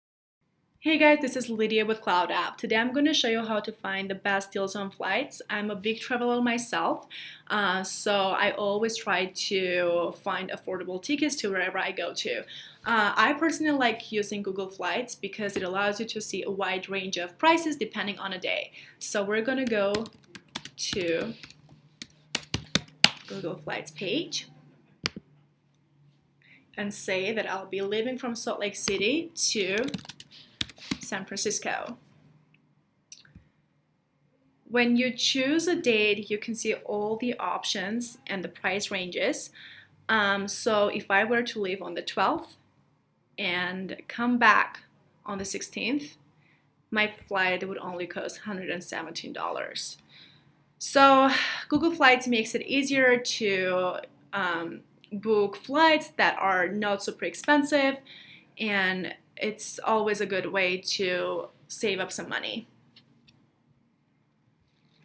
How to Search for Flights Using Google Flights | How to Use the Internet
Learn how to find the cheapest flights using Google Flights. Happy travels!